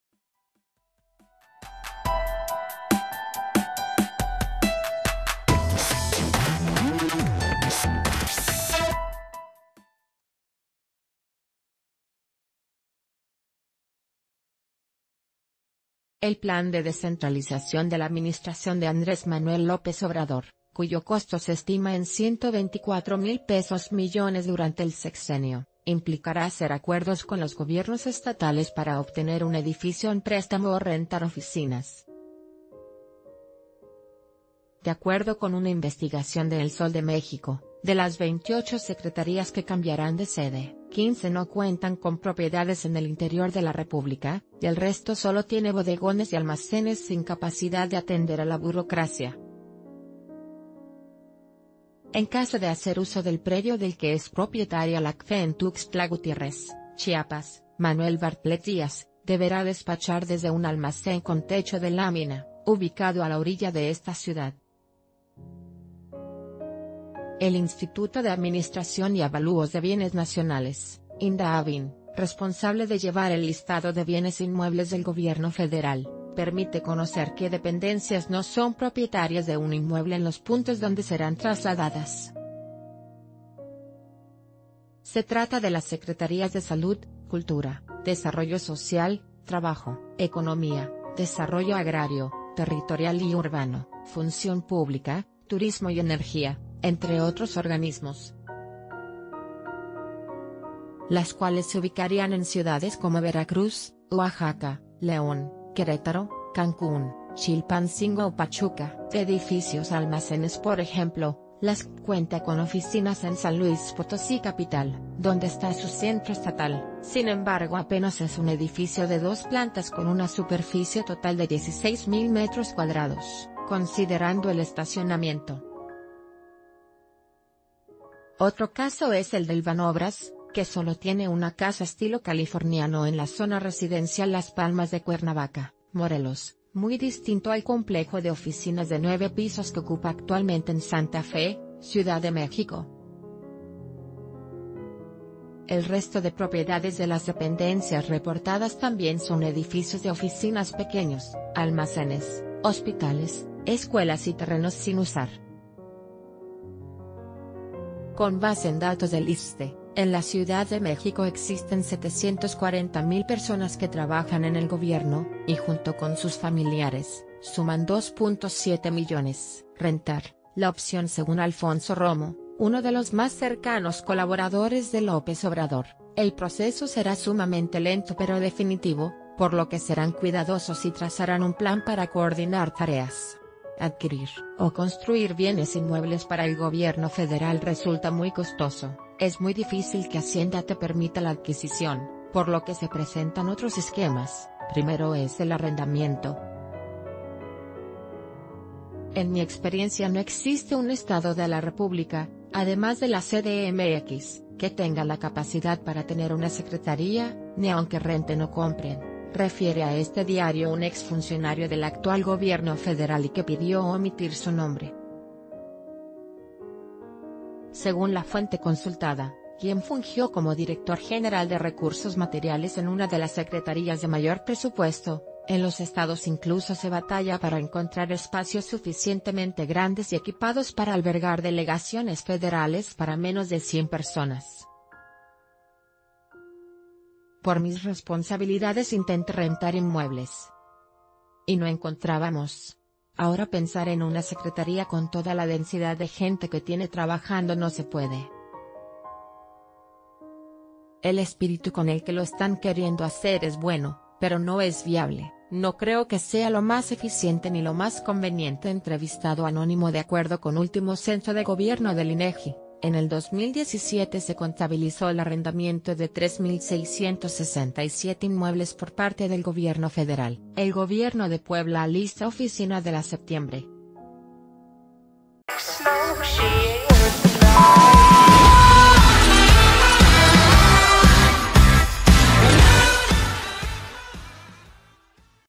En mudanza de secretarías de AMLO, la mayoría operaría en bodegas
El plan de descentralización de la administración de Andrés Manuel López Obrador, cuyo costo se estima en 124 mil pesos millones durante el sexenio, implicará hacer acuerdos con los gobiernos estatales para obtener un edificio en préstamo o rentar oficinas. De acuerdo con una investigación de El Sol...